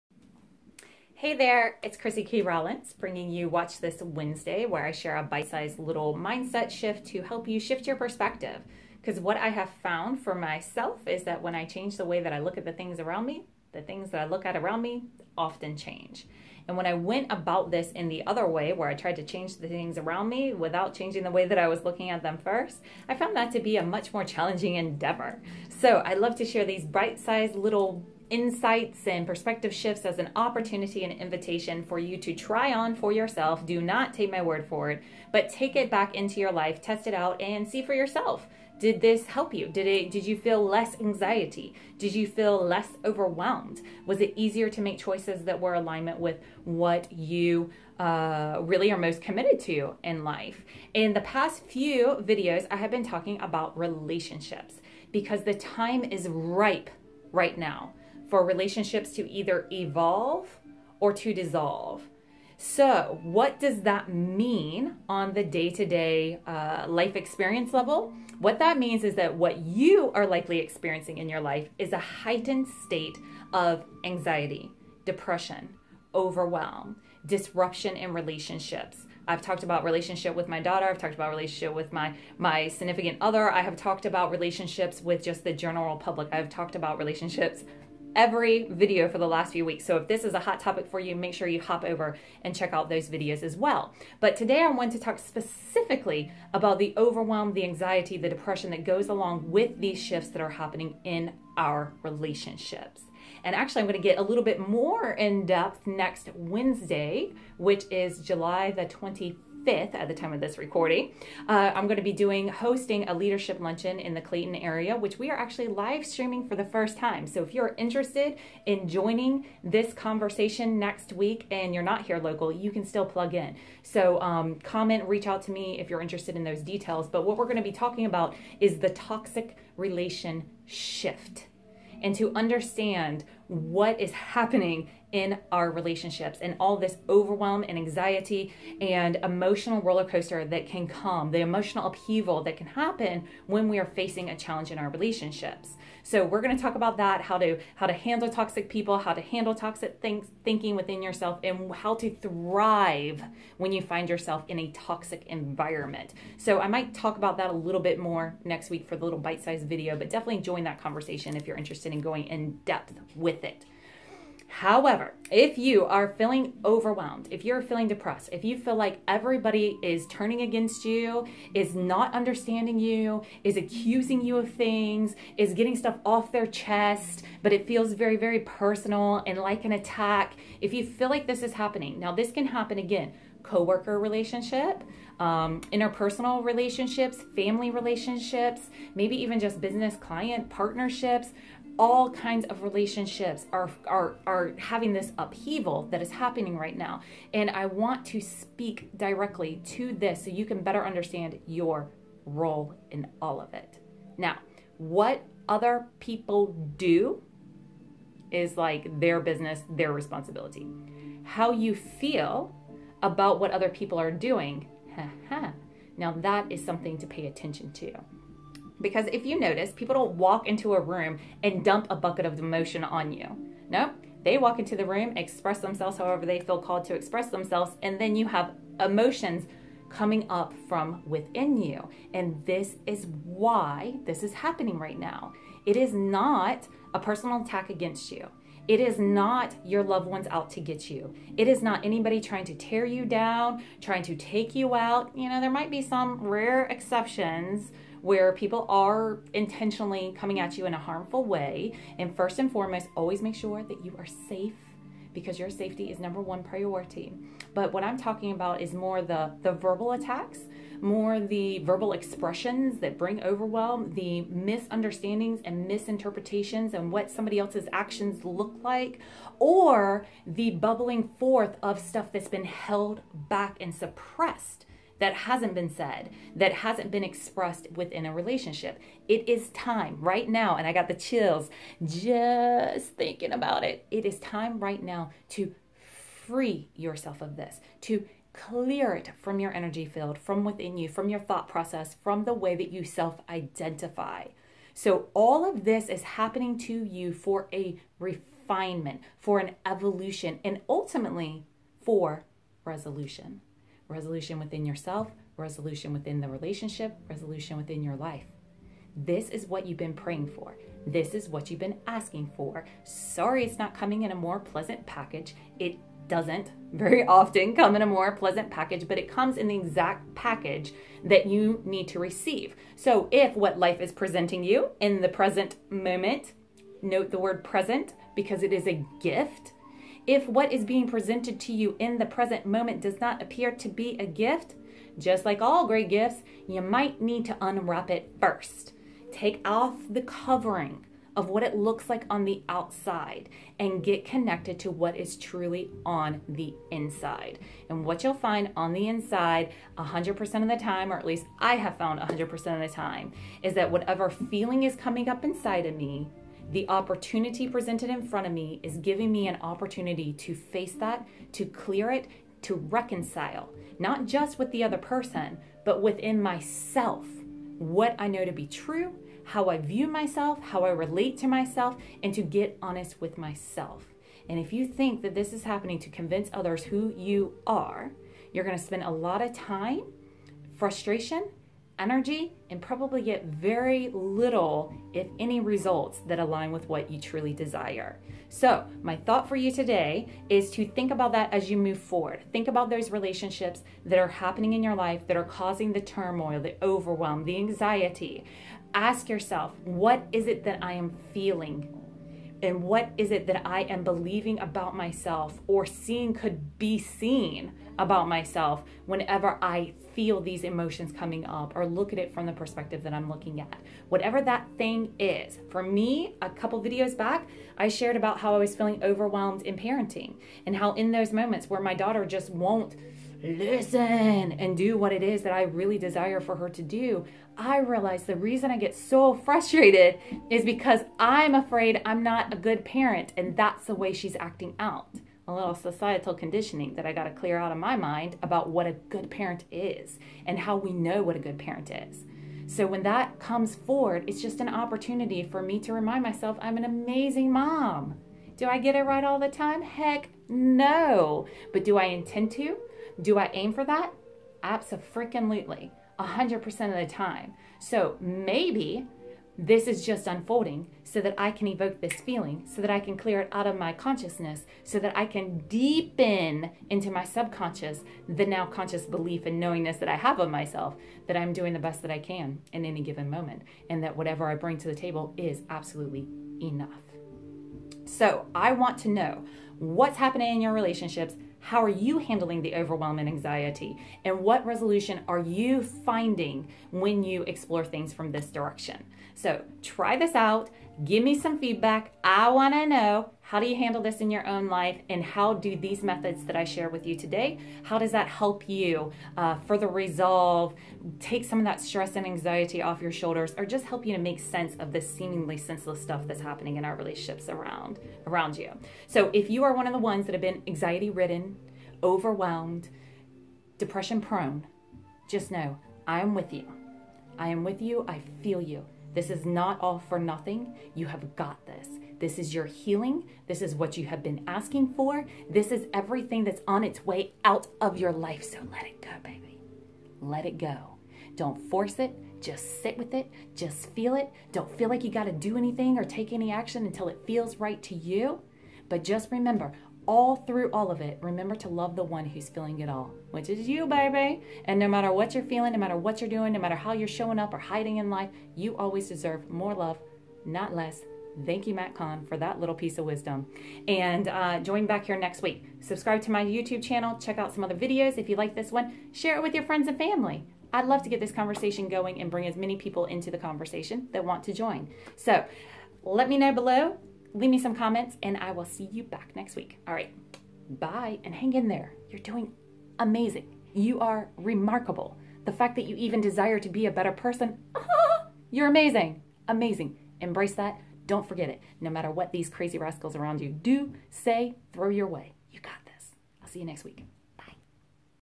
WTF is up with all the overwhelm??
If you've been wishing and praying and hoping for some peace to finally swoop into your life and liberate your suffering, anxiety and overwhelm, then this video is for You! Why is all of this happening? Why is it so overwhelming? Why can't you catch a break? Why's everyone going crazy? Watch this video to find out why and what to do to overcome the overwhelm...